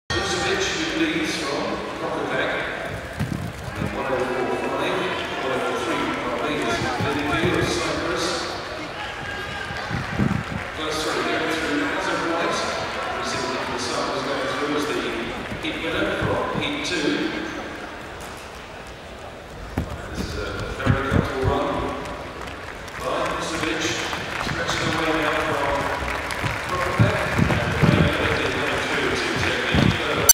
reina in rimonta
La Reina si qualifica giungendo terza